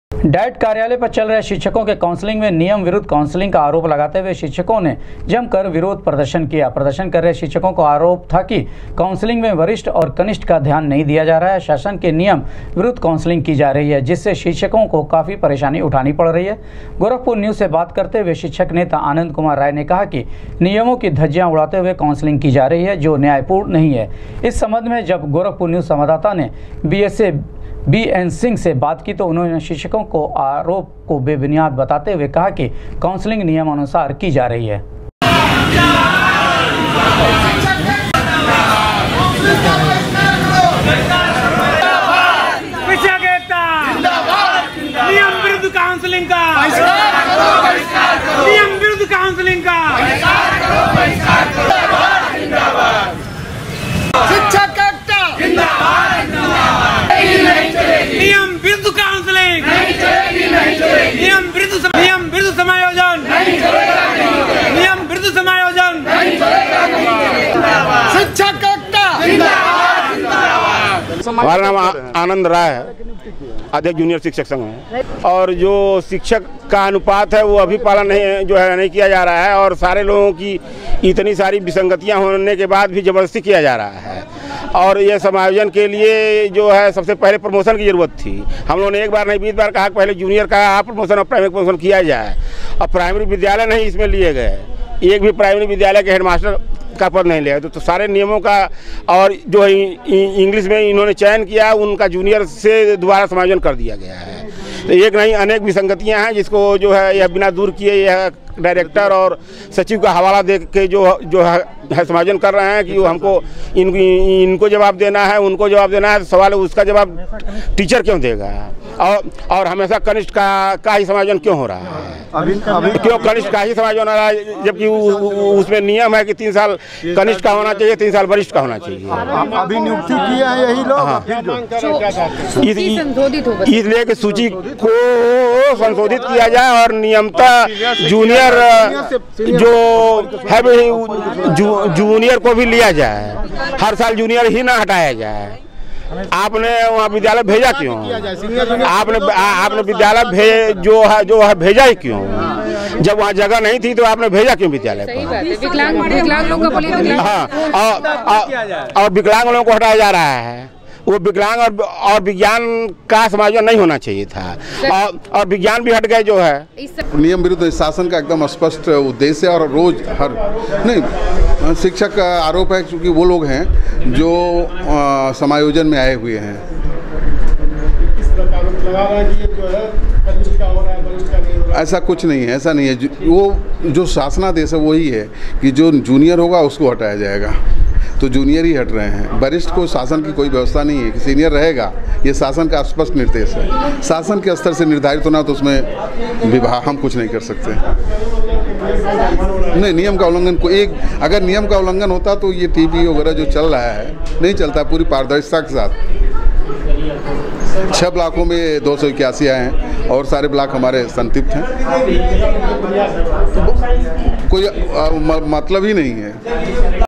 नियम विरुद्ध काउंसिलिंग का आरोप लगाते हुए शिक्षकों ने डायट कार्यालय पर जमकर विरोध प्रदर्शन
नियम विरुद्ध काउंसिलिंग का आरोप लगाते हुए शिक्षकों ने डायट कार्यालय पर जमकर विरोध प्रदर्शन, बीएसए ने कहा शासन के नियमों के आधार पर हो रही है काउंसलिंग